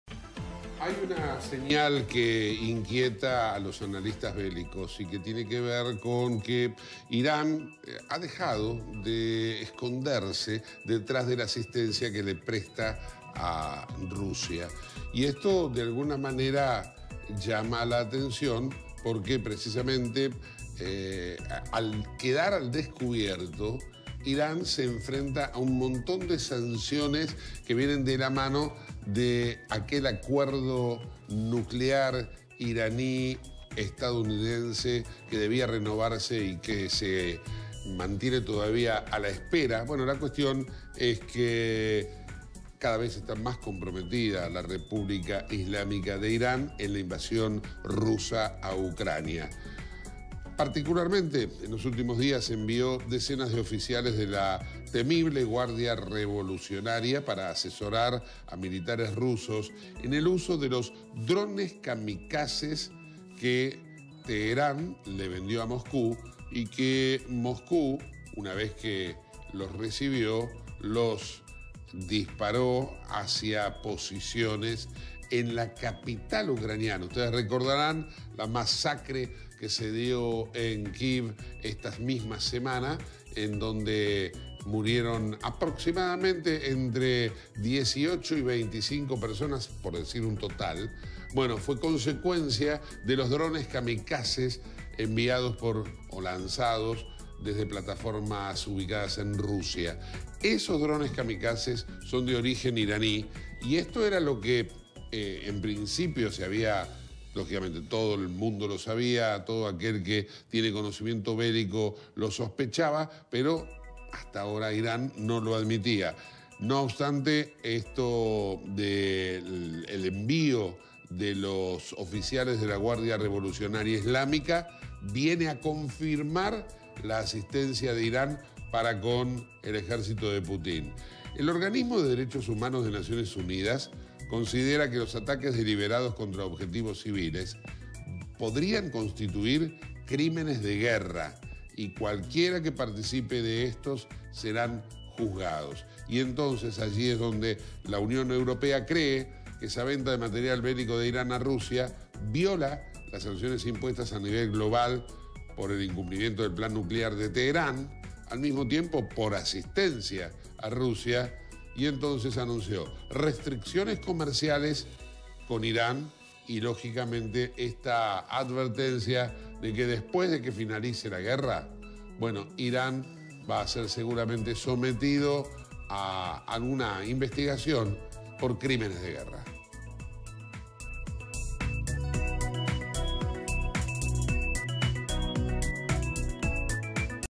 Guardia revolucionaria iraní desembarcó en Crimea
🚨 Los instructores iraníes operan desde una base militar rusa en Crimea, donde se encuentran la mayoría de los aviones no tripulados desde que fueron entregados por Irán y que utilizaron esta semana los rusos para aterrorizar a la población civil ucraniana y destruir centrales eléctricas que dejaron sin suministro a millones de personas.